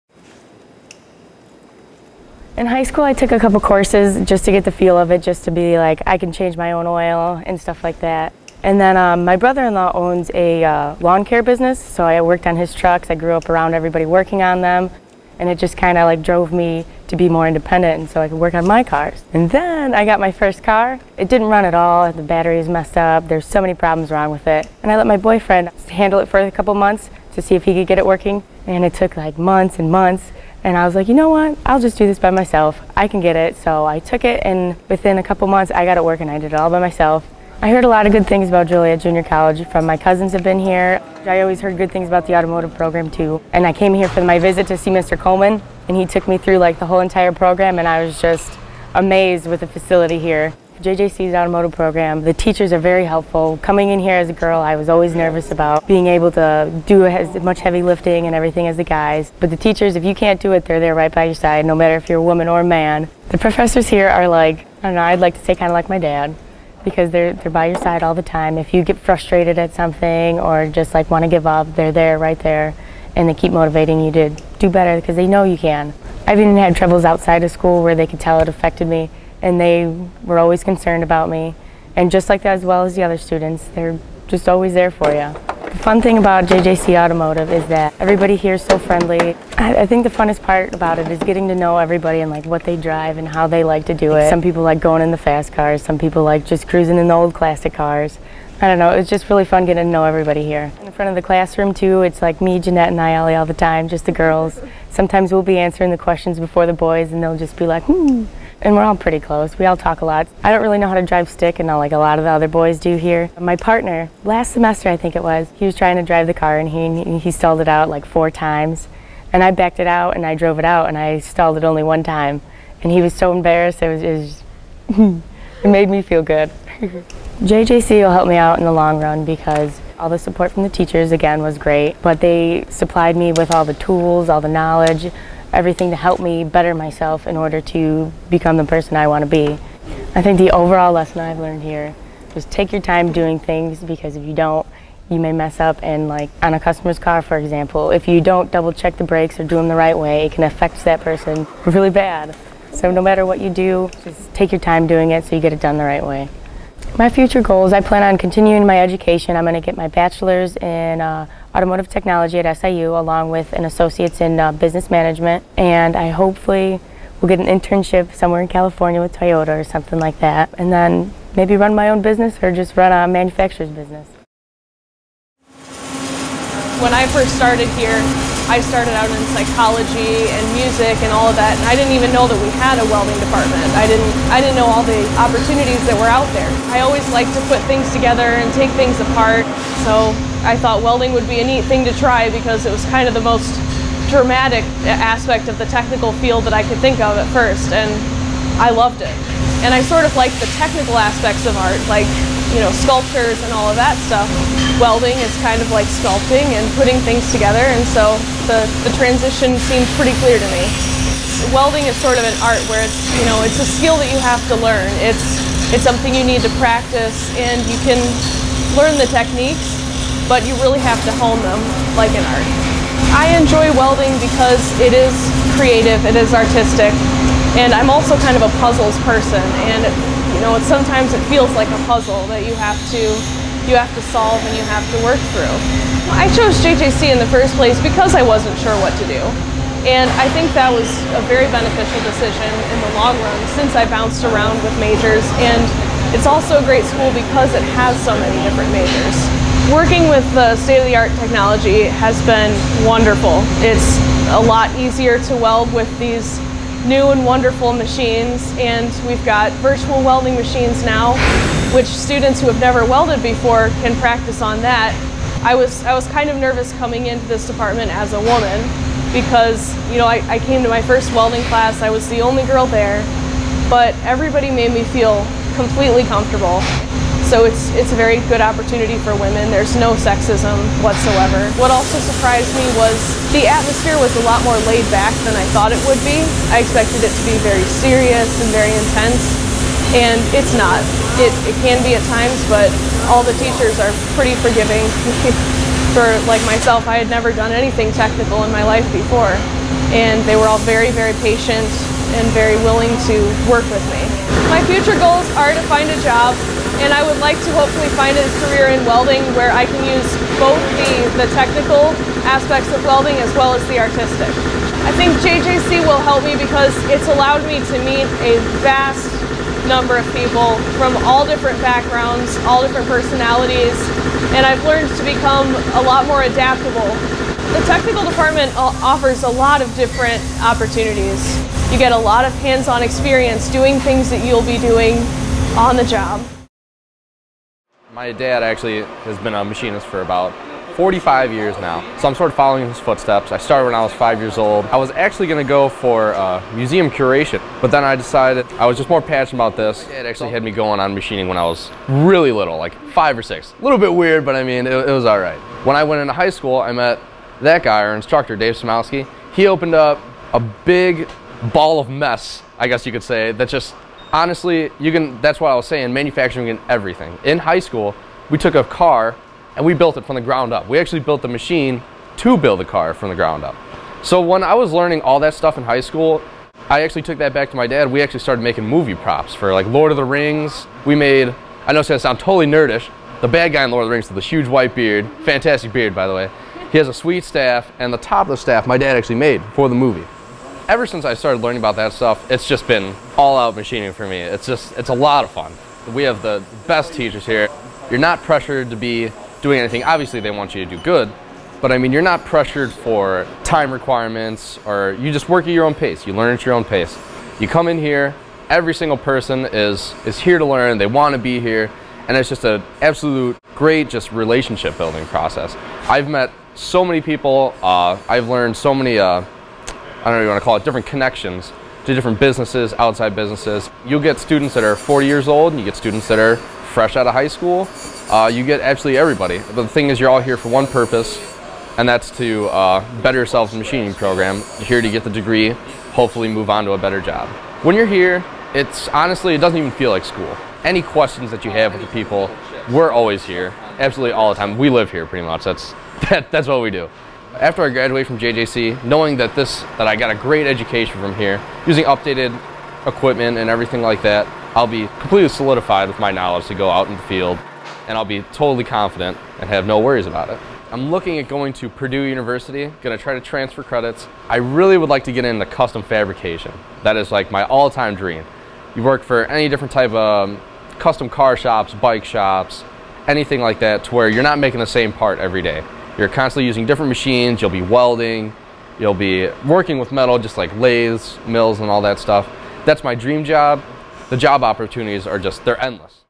JJC Students Express their Passion for the Skilled Trades
Joliet Junior College students Tiffany Greminger, Whitney Wardlow, and Cody Hambly talk about why they chose to study at JJC in their respective fields. Greminger, an automotive alum, found her passion after she decided to fix her first car by herself. Wardlow first came to JJC to study psychology and art before deciding to pursue welding. Hambly got into manufacturing because his dad has been practicing the trade for 45 years.

These three students explain why JJC was the best choice school for them, and why they think the college has great technical programs.